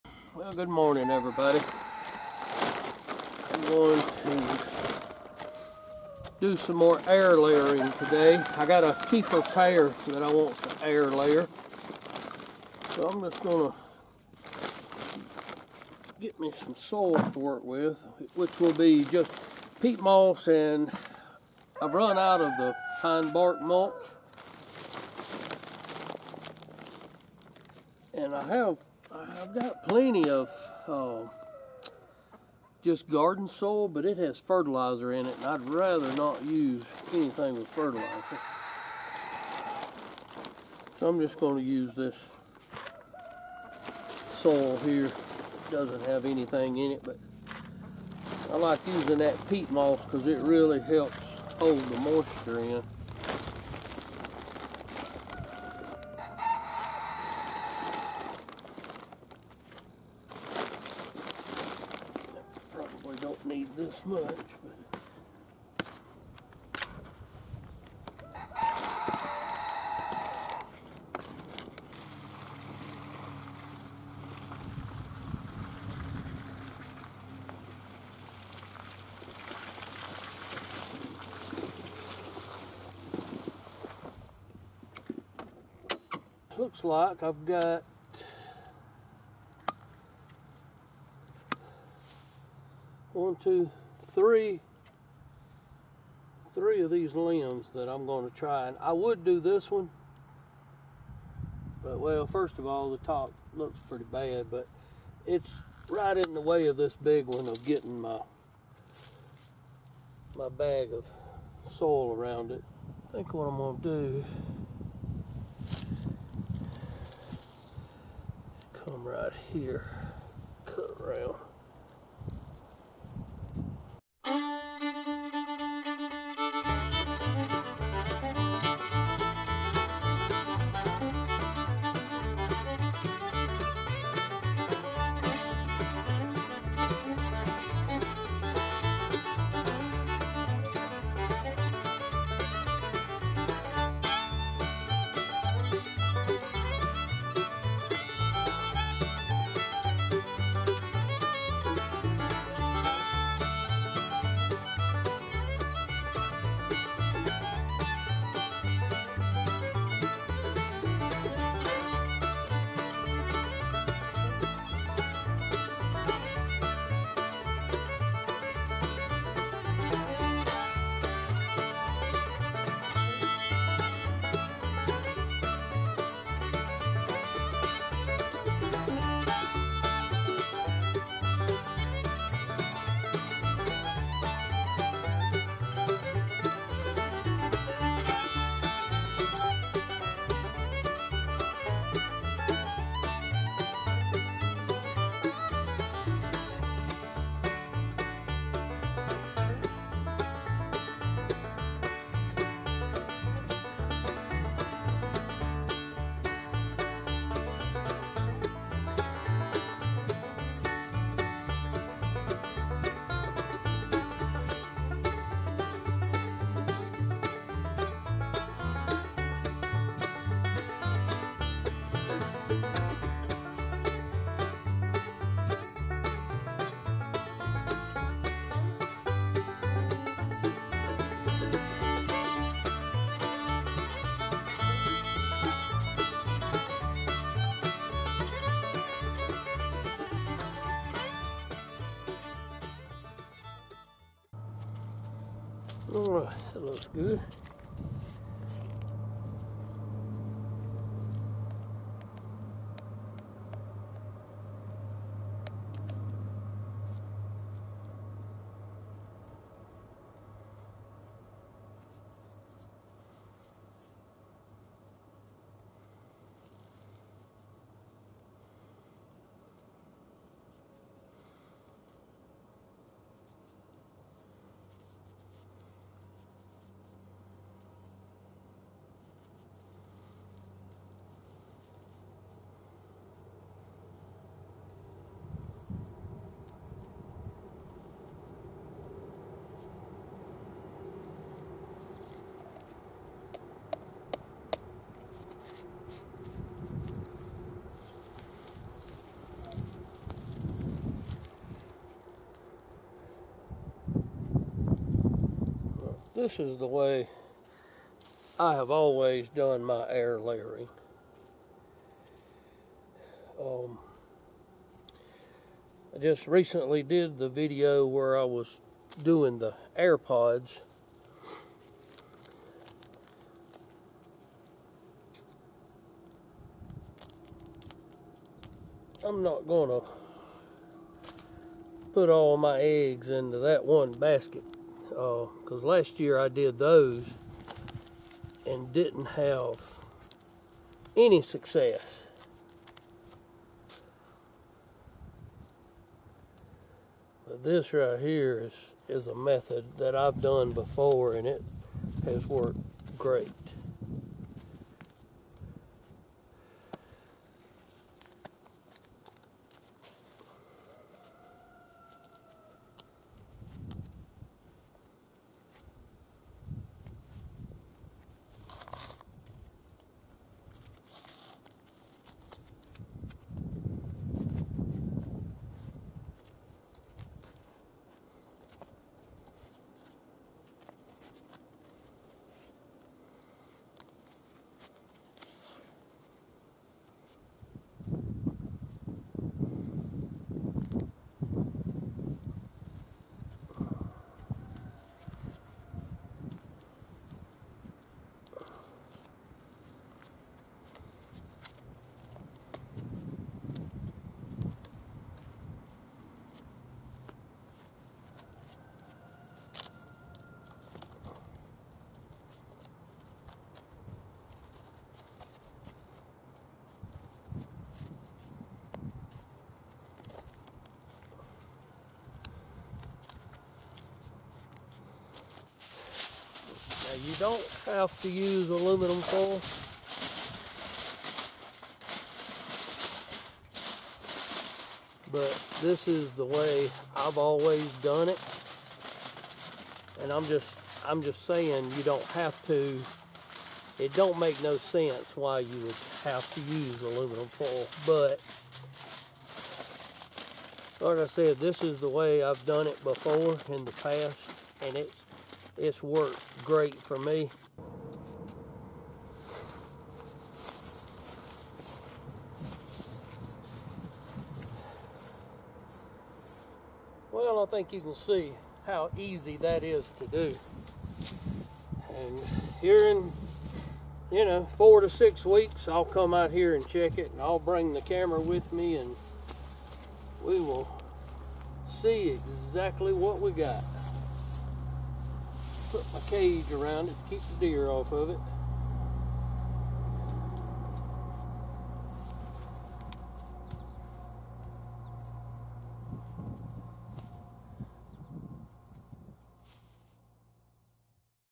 Cloning a Keifer Pear Tree
Tired of paying high prices for fruit trees? Clone fruit trees in a few simple steps